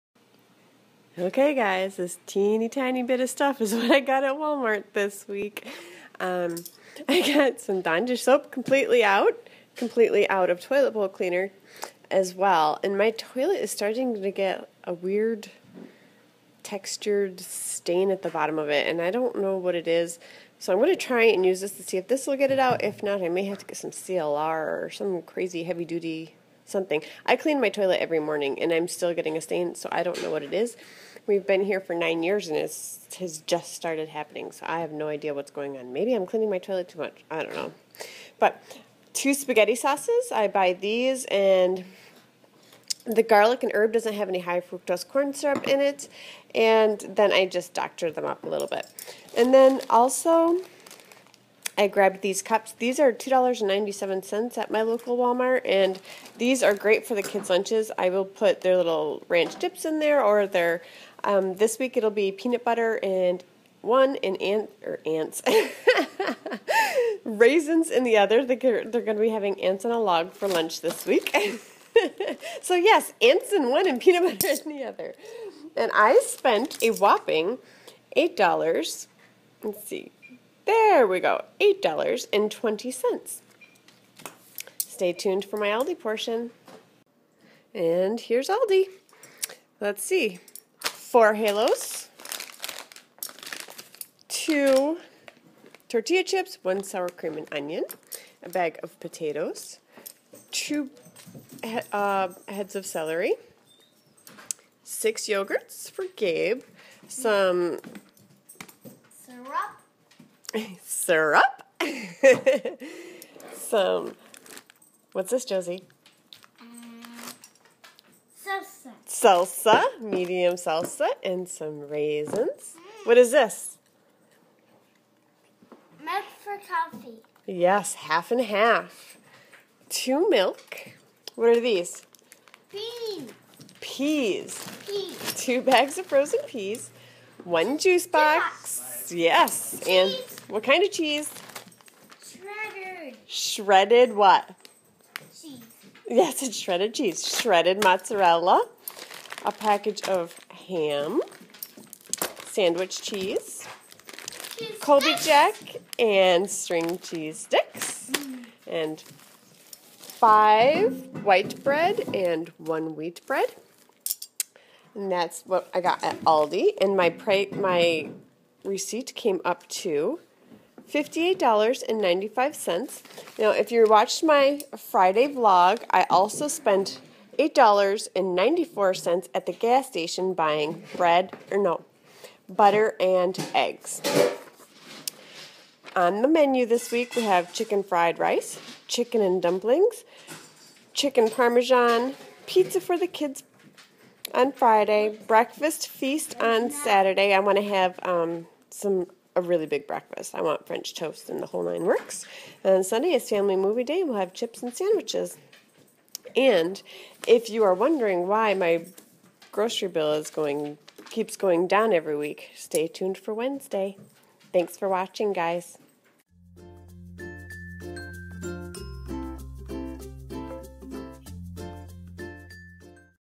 Aldi & Walmart Grocery Haul & Meal Plan 4 20 15
I went to Aldi and Walmart this week. I spent even less this week than last week!